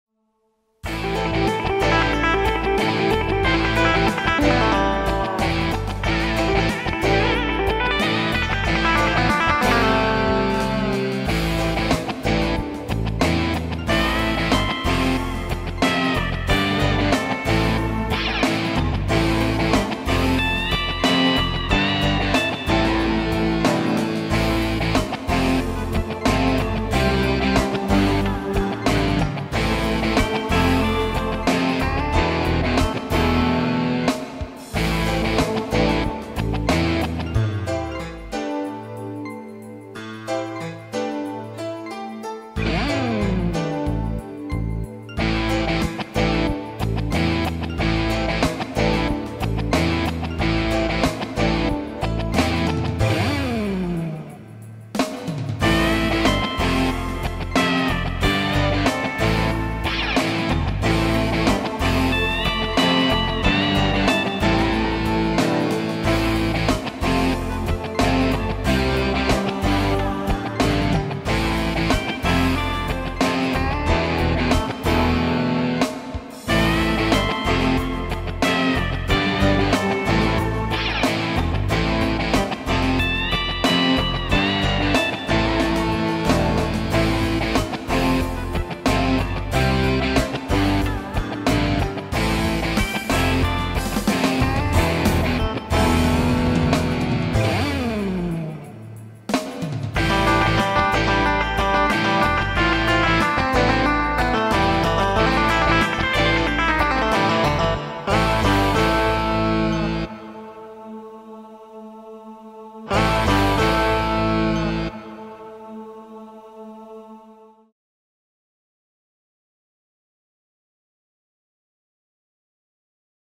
Electric guitars. PSR S 970.2016.
En ce mois de janvier qui se rafraichit,
j'ai un peu la flemme d'improviser,
par contre, je m'amuse bien à musicalement
bricoler sur mon nouveau clavier Yamaha.

Donc, je vais vous proposer quelques " démos "
" d'user styles " modifiés et arrangés sur ce S970...

Voici en premier, celle d'un style nommé 80 S Five Star Pub T 161.

Illustration vidéo :
- Extraits de Clips gratuits et libres d'utilisation. Site Wedistill.io

Merci à toutes et tous.